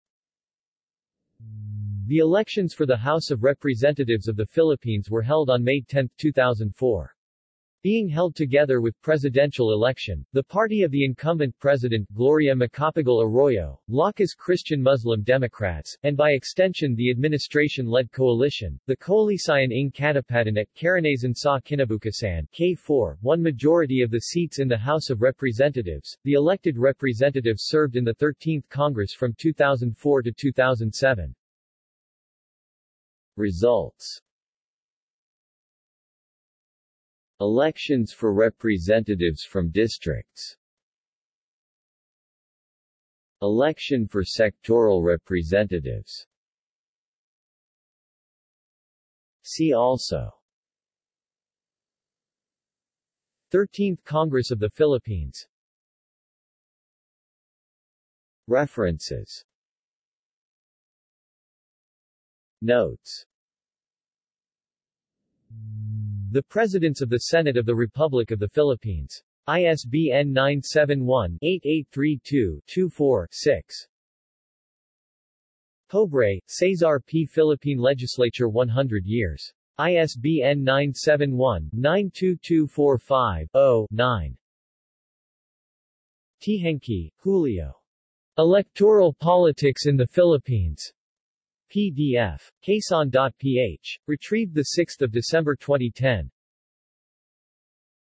Philippine House of Representatives elections, 2004 | Wikipedia audio article
This is an audio version of the Wikipedia Article:
Philippine House of Representatives elections, 2004

SUMMARY
The Elections for the House of Representatives of the Philippines were held on May 10, 2004. Being held together with presidential election, the party of the incumbent president Gloria Macapagal Arroyo, Lakas-Christian Muslim Democrats, and by extension the administration-led coalition, the Koalisyon ng Katapatan at Karanasan sa Kinabukasan (K4), won majority of the seats in the House of Representatives.The elected representatives served in the 13th Congress from 2004 to 2007.